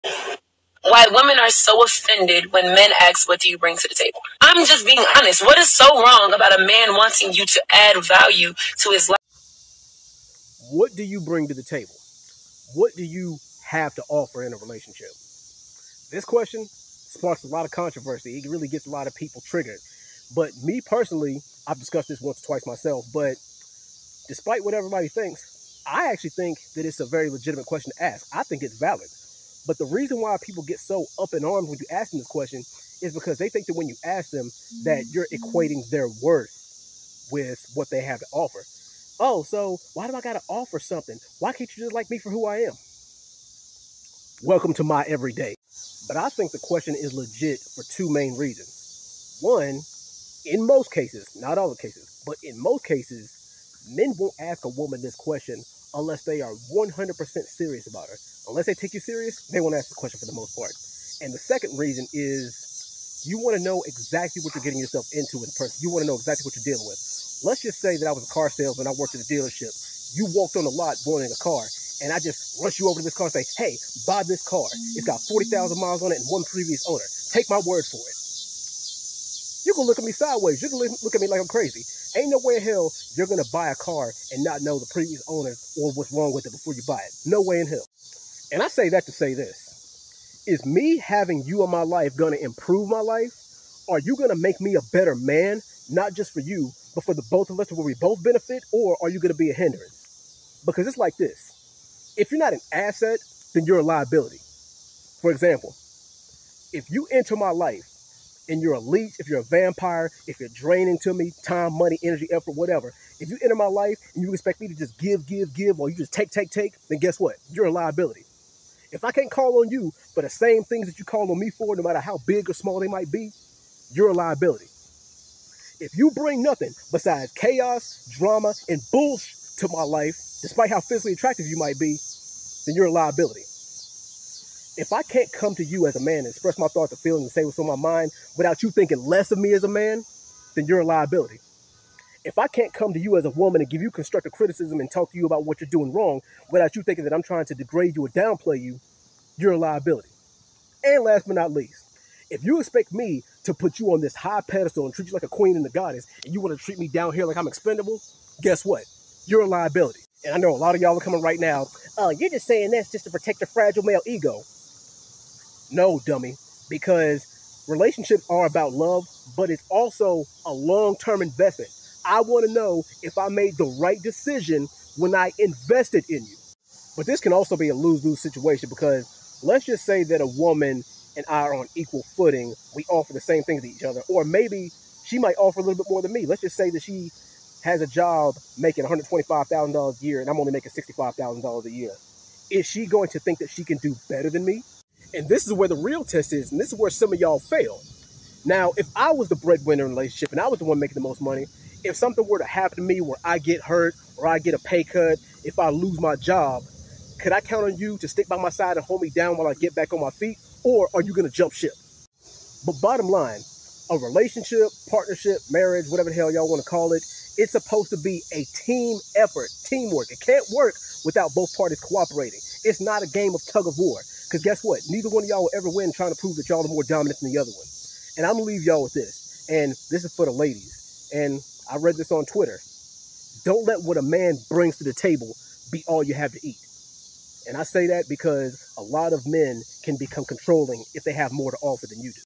why is "what you bring to the table?" such a triggering question
in modern day relationships equality has been pushed to the highest peak but yet there's a triggering question that gauges a lot of negative responses and that question is what do you have to offer / bring to the table?